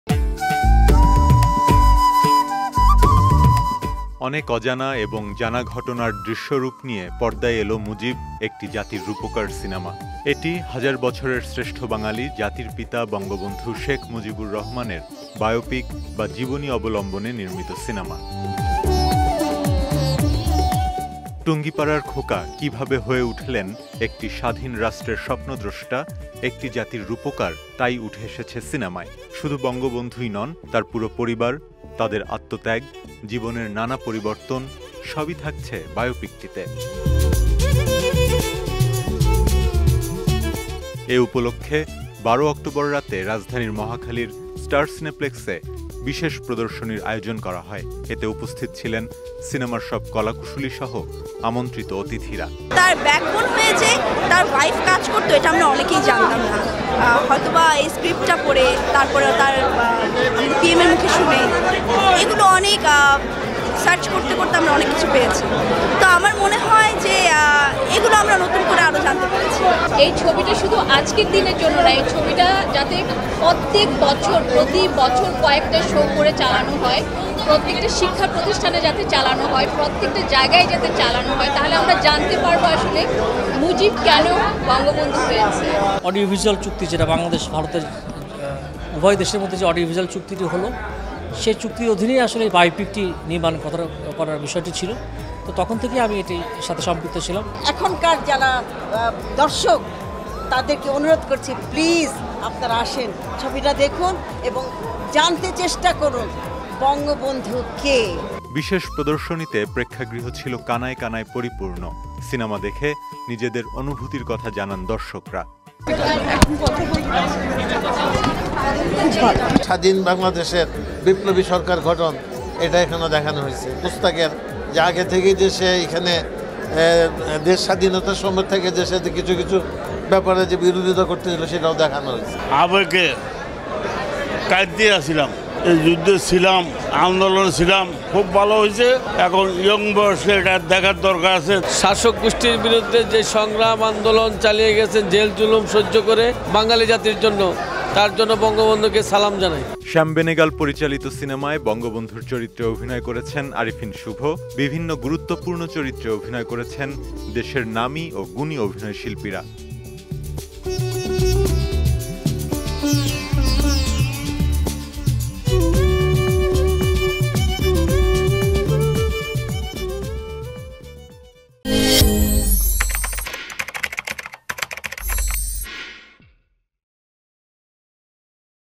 ‘মুজিব’ সিনেমা দেখে আপ্লুত দর্শক Mujib। Mujib: The Making of a Nation। Bijoy TV
অনেক অজানা এবং জানা ঘটনার দৃশ্যরূপ নিয়ে পর্দায় এল মুজিব: একটি জাতির রূপকার সিনেমা। এটি হাজার বছরের শ্রেষ্ঠ বাঙালি, জাতির পিতা বঙ্গবন্ধু শেখ মুজিবুর রহমানের বায়োপিক বা জীবনী অবলম্বনে নির্মিত সিনেমা।
টুঙ্গিপাড়ার খোকা কীভাবে হয়ে উঠলেন একটি স্বাধীন রাষ্ট্রের স্বপ্নদ্রষ্টা-একটি জাতির রূপকার, তাই উঠে এসেছে সিনেমায়। শুধু বঙ্গবন্ধুই নন, তার পুরো পরিবার, তাদের আত্মত্যাগ, জীবনের নানা পরিবর্তন, সবই থাকছে বায়োপিকটিতে।
সিনেমাটি মুক্তি পাচ্ছে ১৩ অক্টোবর। এ উপলক্ষে ১২ অক্টোবর রাতে রাজধানীর মহাখালীর স্টার সিনেপ্লেক্সে বিশেষ প্রদর্শনীর আয়োজন করা হয়। এতে উপস্থিত ছিলেন, সিনেমার সব কলাকুশলীসহ আমন্ত্রীত অতিথিরা।
বিশেষ প্রদর্শনীতে প্রেক্ষাগৃহ ছিল কানায় কানায় পরিপূর্ণ। সিনেমা দেখে নিজেদের অনুভূতির কথা জানান দর্শকরা।
শ্যাম বেনেগাল পরিচালিত সিনেমায় বঙ্গবন্ধুর চরিত্রে অভিনয় করেছেন আরিফিন শুভ। বিভিন্ন গুরুত্বপূর্ণ চরিত্রে অভিনয় করেছেন দেশের নামি ও গুণী অভিনয়শিল্পীরা।